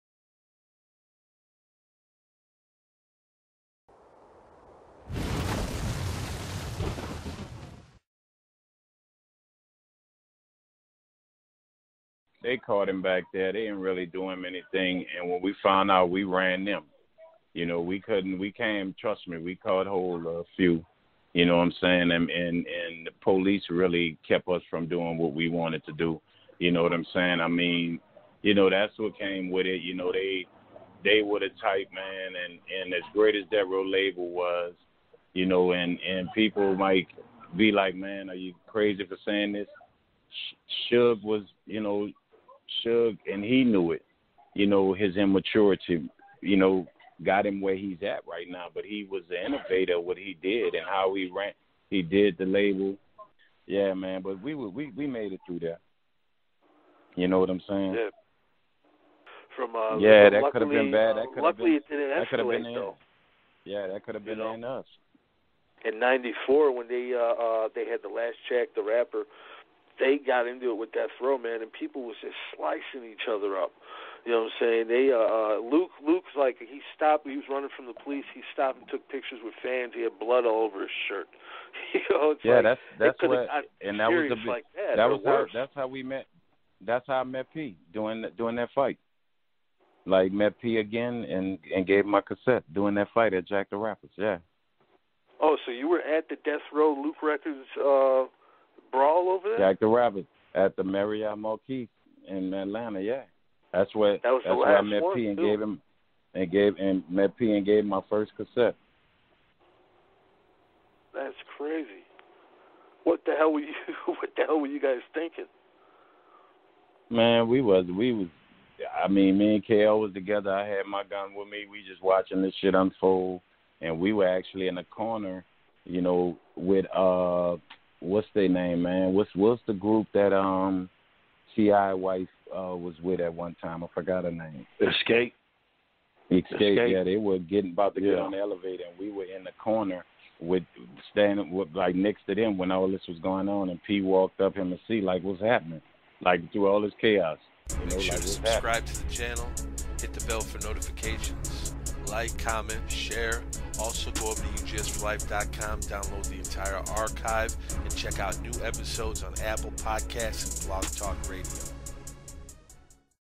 Mr. Serv-On recalls '98 Snoop and Death Row altercation also '94 brawl at the last Jack The Rapper
Check out the entire interview and visit the archive of over 731 shows